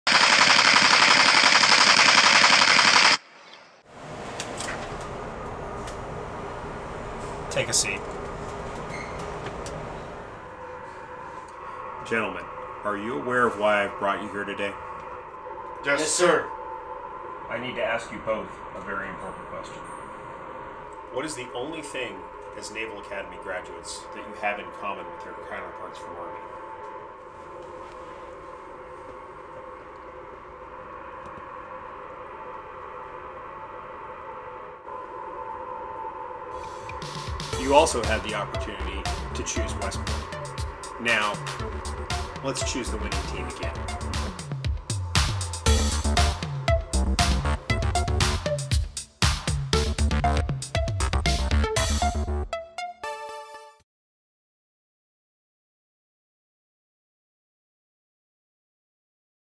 Decisions Make the Difference: Go Navy, Beat Army
USS Emory S. Land, where pride never sleeps, looks for the commonalities between Naval Academy graduates and their counterparts from West Point.